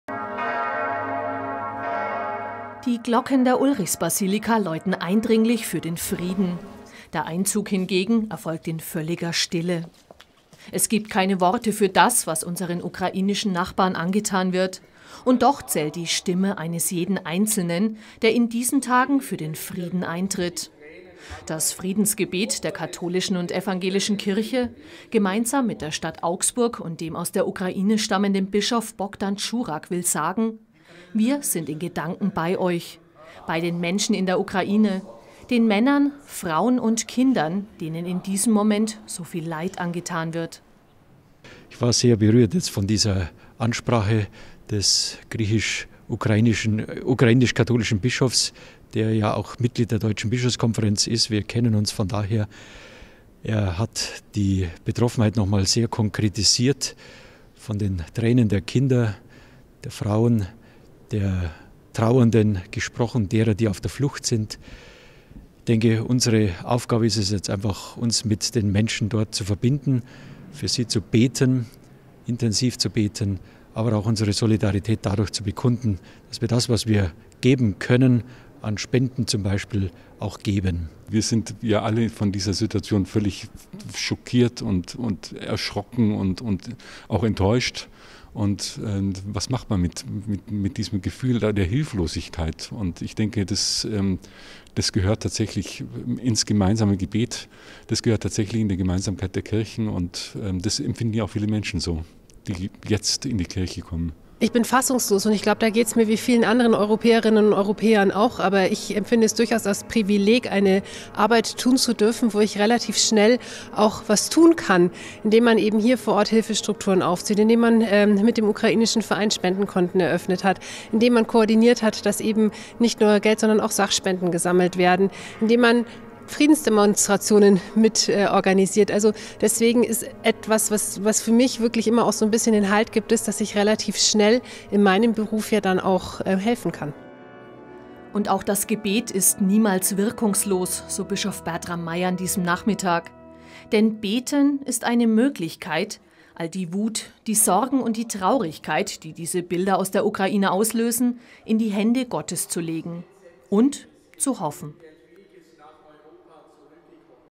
Friedensgebet in der Ulrichsbasilika
Mit einem Friedensgebet in der Augsburger Basilika St. Ulrich und Afra haben in dieser Woche die katholische und evangelische Kirche, gemeinsam mit der Stadt Augsburg und dem aus der Ukraine stammenden Bischof Bohdan Dzyurakh auf die Not der Menschen in der Ukraine aufmerksam gemacht und für sie gebetet.